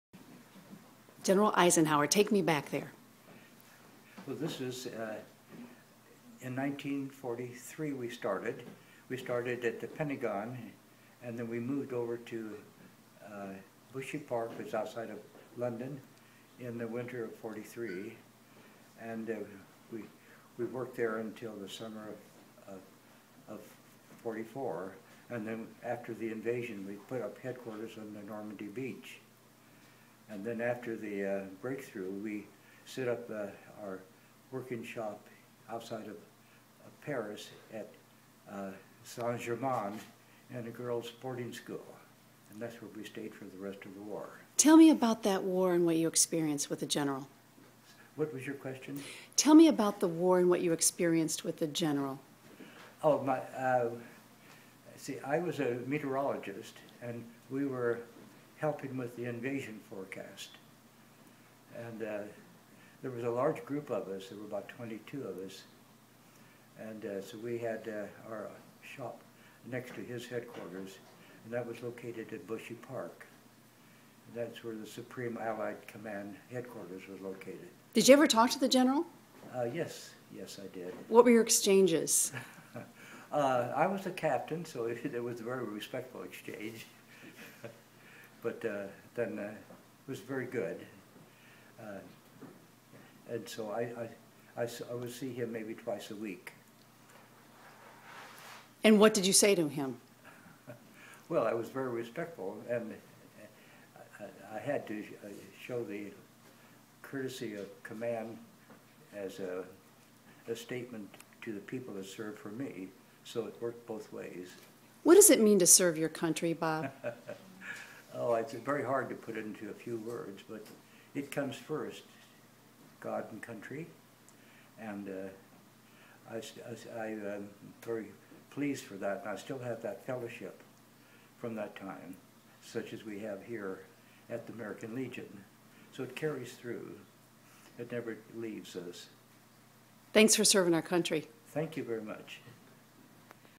Robert C. Bundgaard, Veteran of World War II
Ninety-four year old Robert C. Bundgaard was presented with a Quilt of Valor in Madison, CT by the women of Quilts of Valor Foundation Bundgaard served alongside General Eisenhower as a meteorologist in World War II.